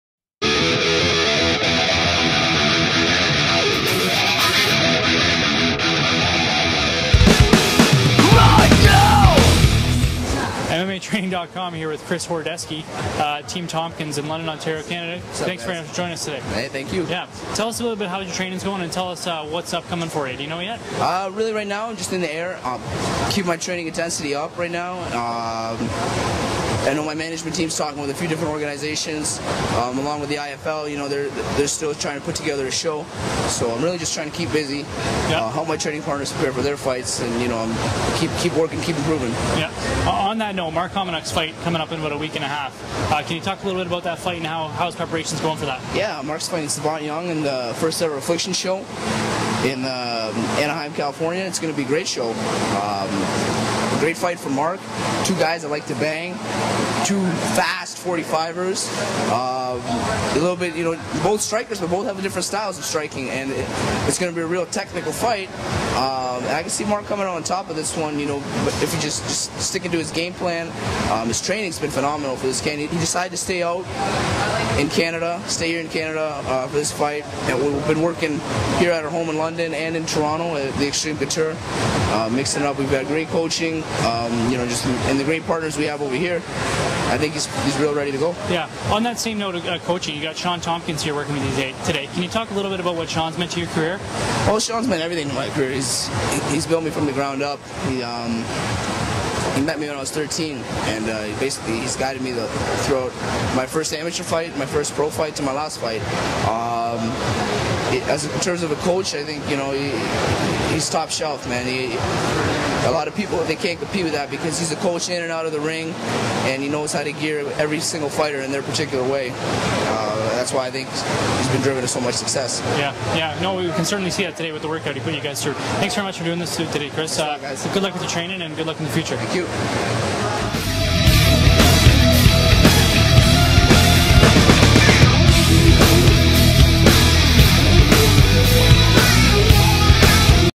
Chris Horodecki speaks with MMATraining.com
MMA star Chris Horodecki spoke with MMATraining.com on July 9, 2008 during a visit to Team Tompkins in London, Ontario, Canada.

Chris Horodecki is currently fighting as a lightweight for the Los Angeles Anacondas of the International Fight League (IFL).

Chris speaks with MMATraining.com about his current training and his training partner Mark Hominick's upcoming fight at Affliction: Banned on July 19th.